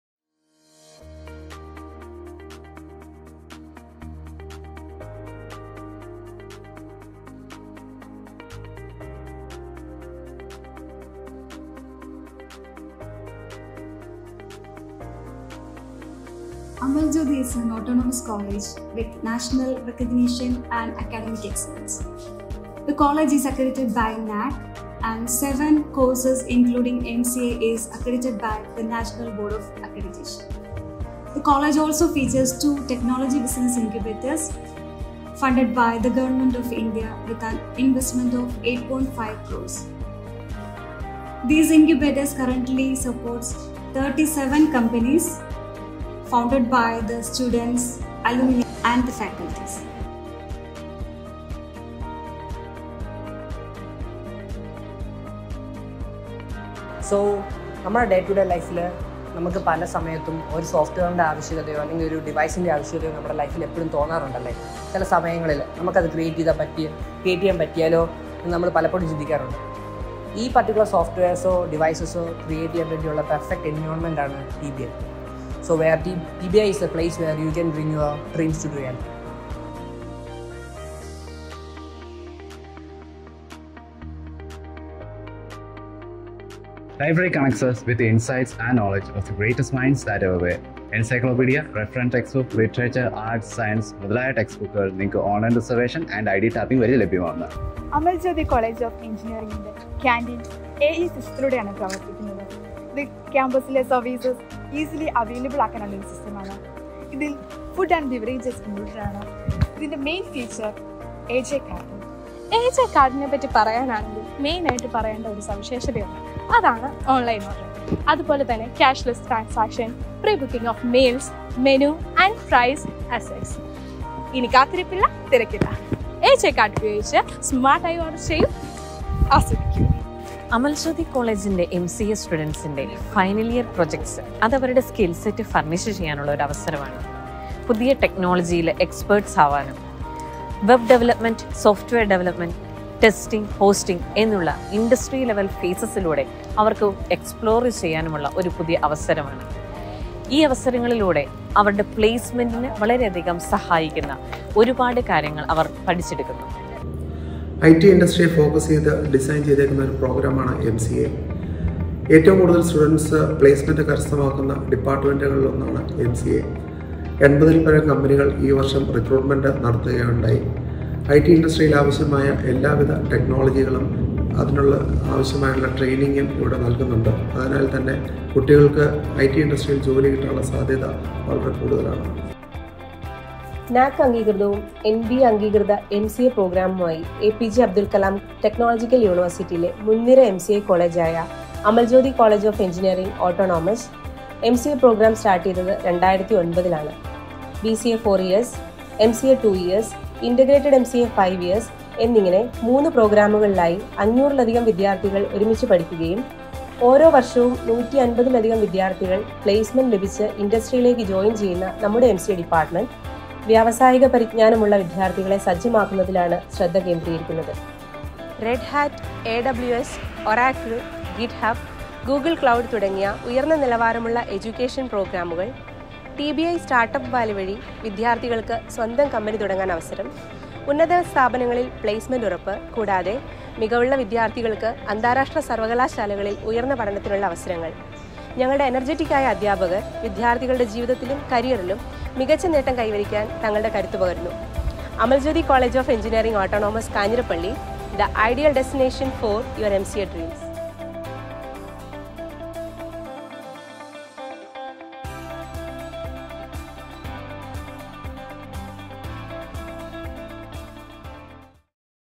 Discover Your Future in Tech | BCA, MCA & Integrated MCA @ Amal Jyothi College of Engineering
Step into the world of innovation and opportunity with the Department of Computer Applications at Amal Jyothi College of Engineering Autonomous. 🌟
We offer:
✅ Bachelor of Computer Applications (BCA)
✅ Master of Computer Applications (MCA)
✅ Integrated MCA (5-Year Program)

🔹 Industry-aligned curriculum
🔹 Experienced faculty & state-of-the-art labs
🔹 Strong placement support
🔹 Holistic development through tech fests, hackathons & more

Join a vibrant learning community and shape your future in software development, AI, data science, and emerging tech fields.